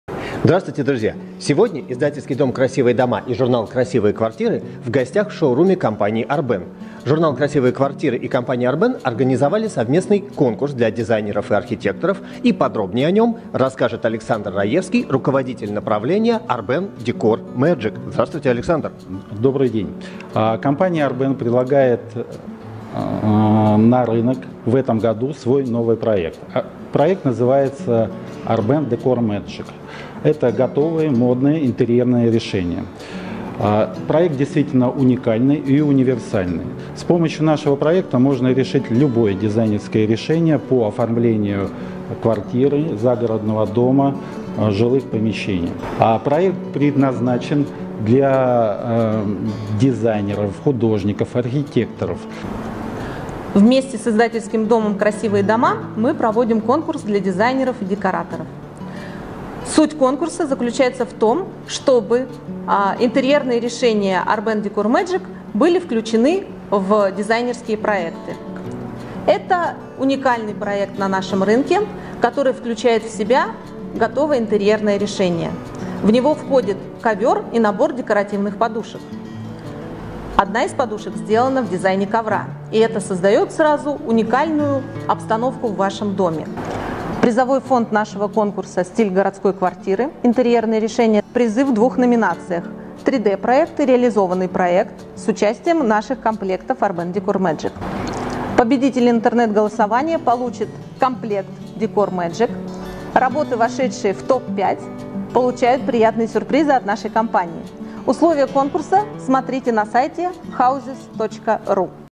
Интерьерный конкурс от компании Arben
Выиграй путешествие или контракт на проект!

Гран-при конкурса «Стиль городской квартиры: интерьерные решения для ARBEN.DECOR MAGIC» — сертификат на 50 тыс. руб. на туристические услуги. С победителями в номинациях конкурса заключаются договоры на разработку дизайн-проекта интерьера стоимостью 20 000 рублей каждый. Страница конкурса в интернете